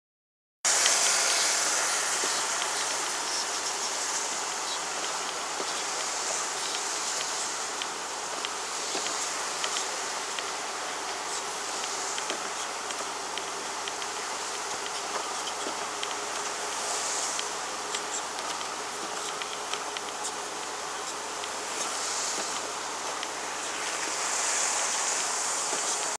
Its a snow day
driving during a snowstorm, January 2013